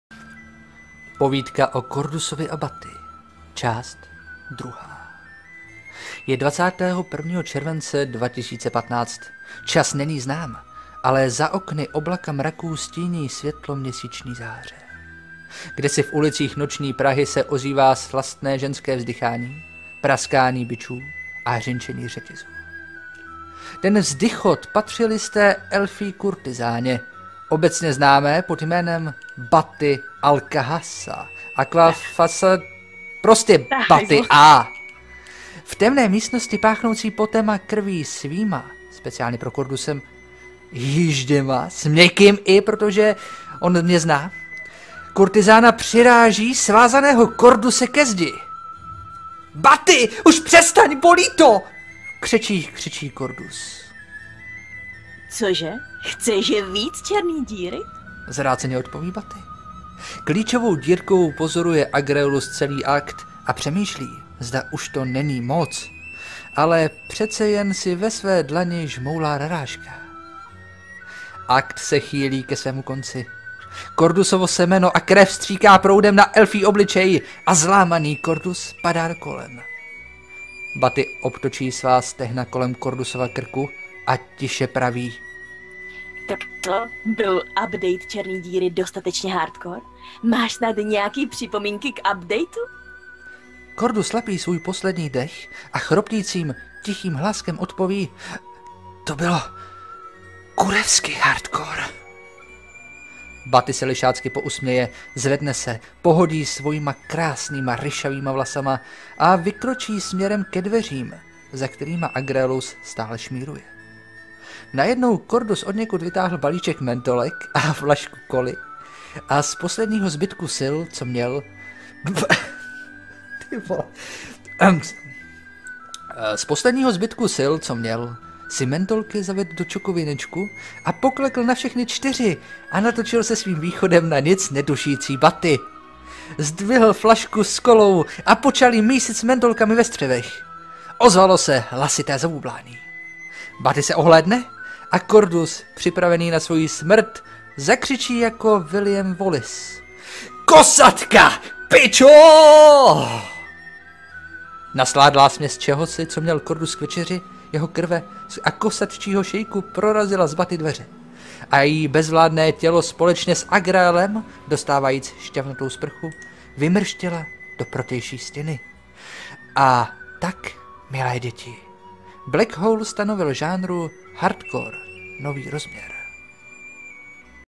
► Papa Kordus vypráví povídky | #4 | Prostě Baty A.
Speciální povídka, která je zpestřena samotnou slavnou Baty. A jak jinak samozřejmě trochu perverzní :D